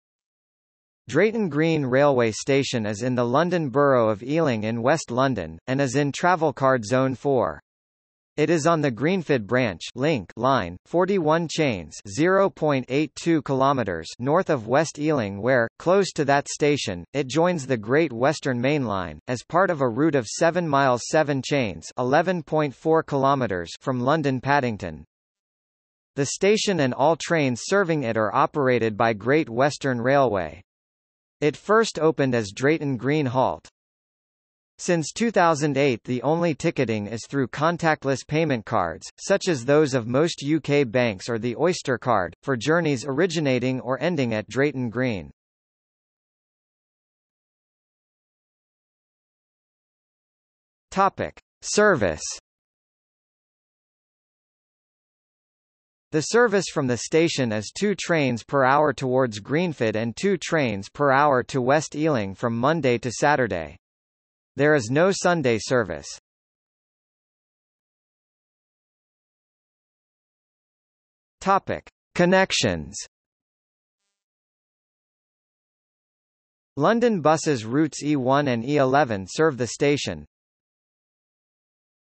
Drayton Green railway station | Wikipedia audio article
00:00:47 1 Service
00:01:07 2 Connections

- Socrates

SUMMARY
Drayton Green railway station is in the London Borough of Ealing in west London, and is in Travelcard Zone 4. It is on the Greenford Branch (Link) Line: 41 chains (0.82 km) north of West Ealing where, close to that station, it joins the Great Western mainline, as part of a route of 7 miles 7 chains (11.4 km) from London Paddington.
The station and all trains serving it are operated by Great Western Railway. It first opened as Drayton Green Halt.
Since 2008 the only ticketing is through contactless payment cards, such as those of most UK banks or the Oyster card, for journeys originating or ending at Drayton Green.